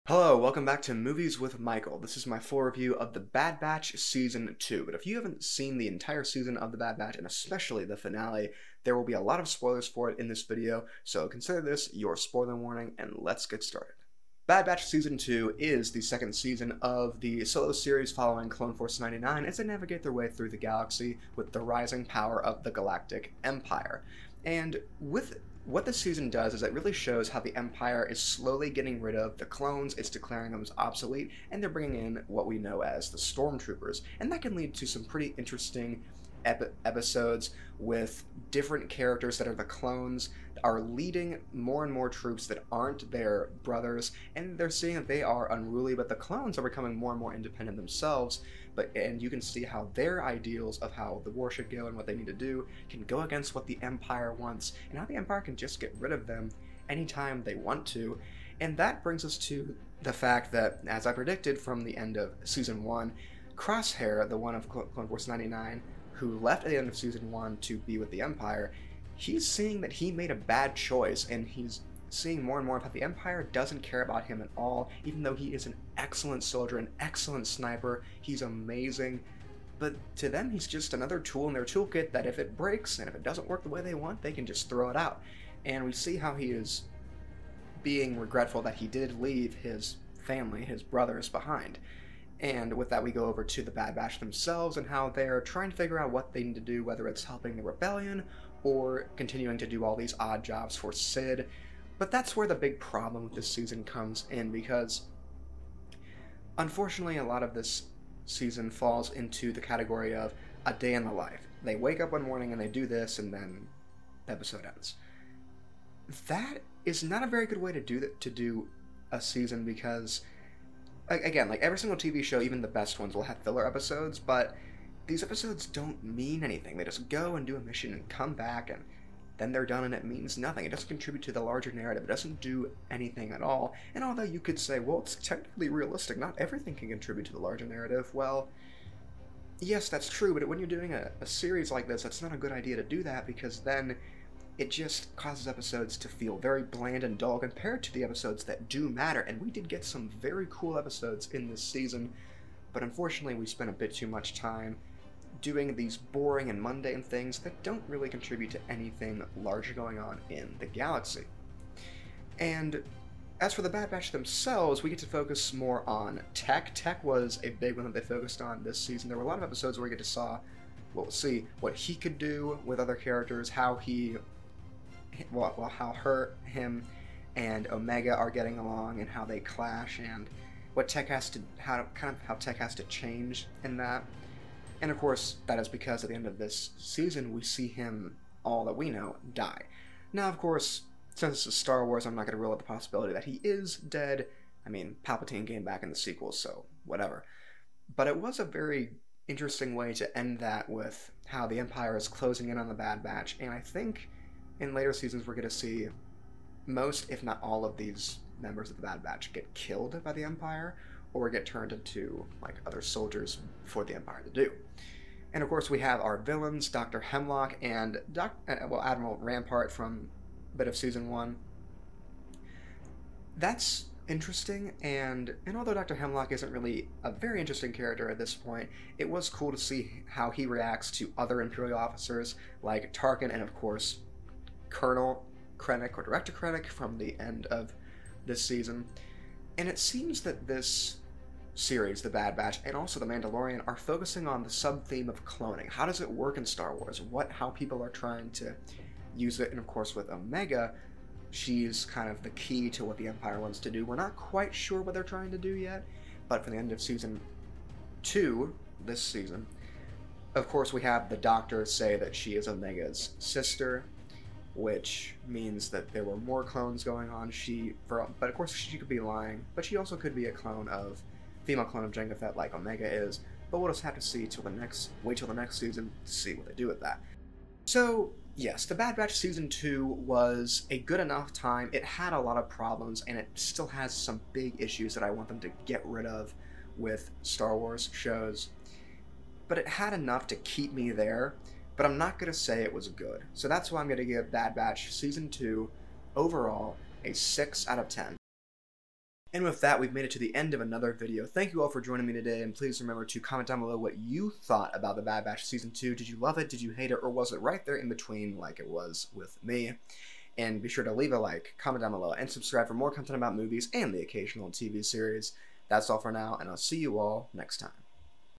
The Bad Batch Season 2: Series Review
The second season of the solo Disney Plus Show following "The Bad Batch" has concluded. I am a big fan of the Clone Wars Show, but my opinions on the other animated Star Wars shows have been less and less positive. Did this season change anything for me? Find out my full review now!